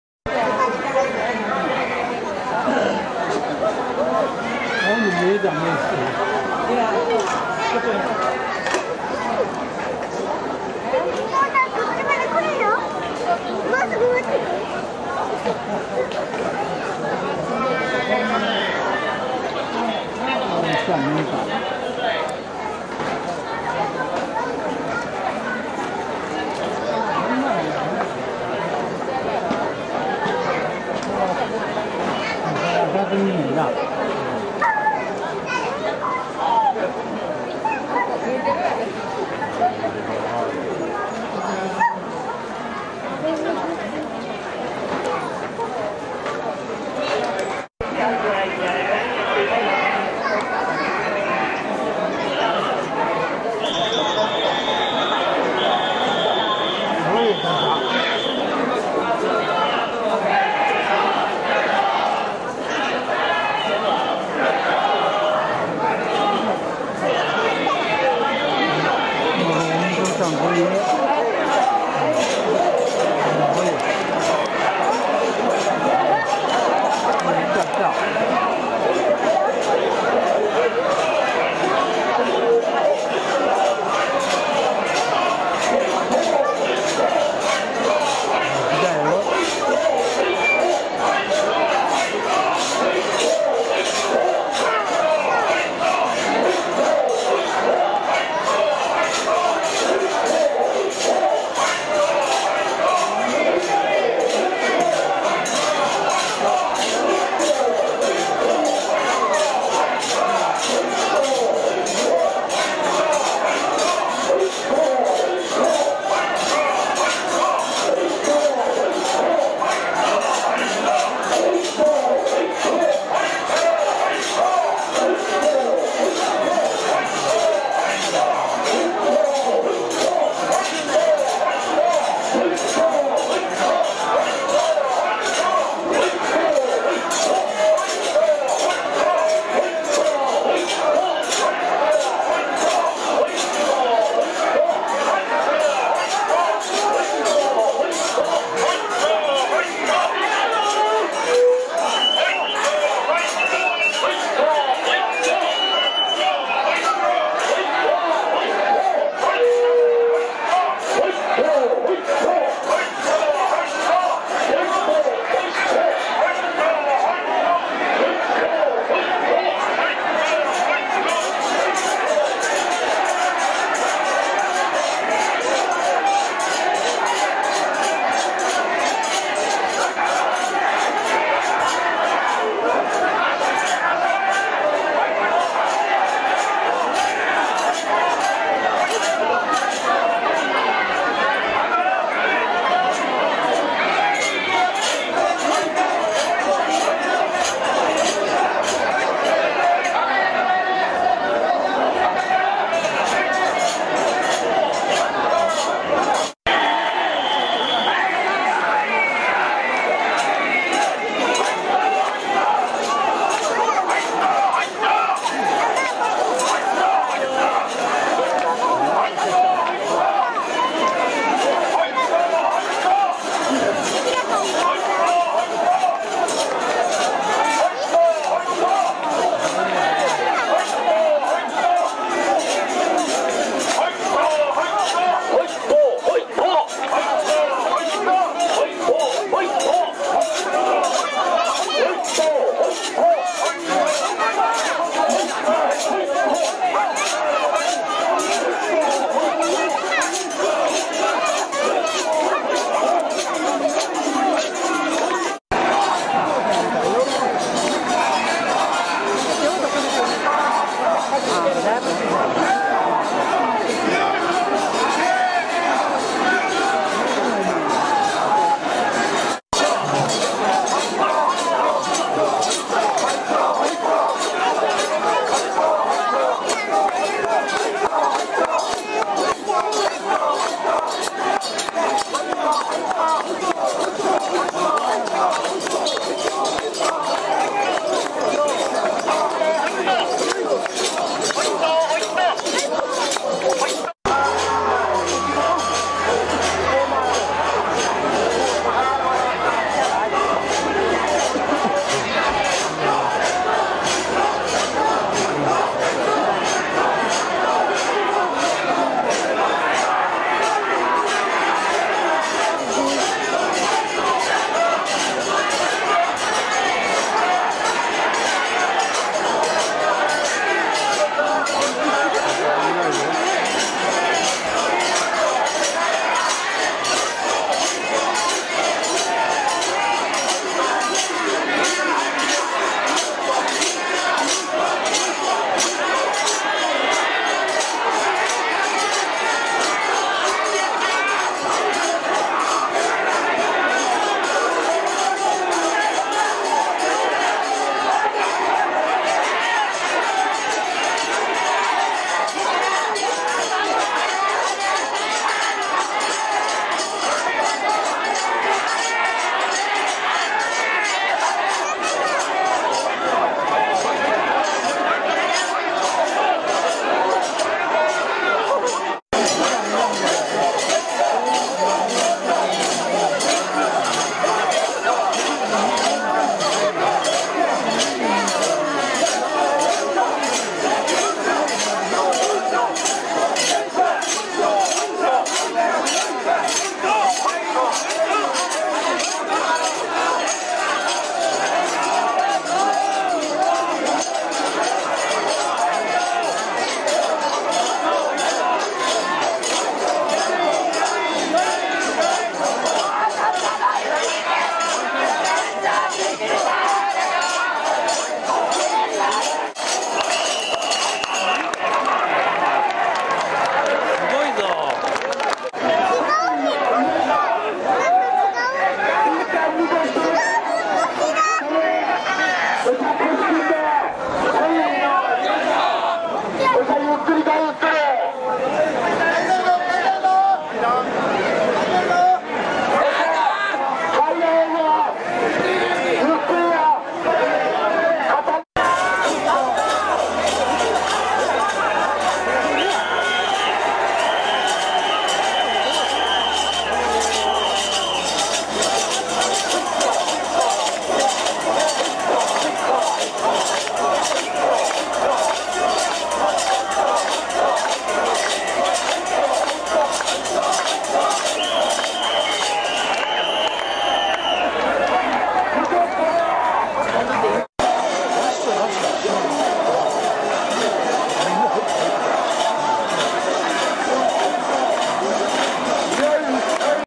松尾大社「還幸祭（おかえり）」拝殿廻しダイジェスト版2012年
松尾大社の「還幸祭」です。通称「おかえり」と呼ばれて親しまれています。拝殿をぐるぐると三回廻す「拝殿廻し」の後に神輿から御本殿分霊の御神体を還御します。各御神輿の個別動画もアップしてありますが、これはまとめて見れるように全部をつないであります。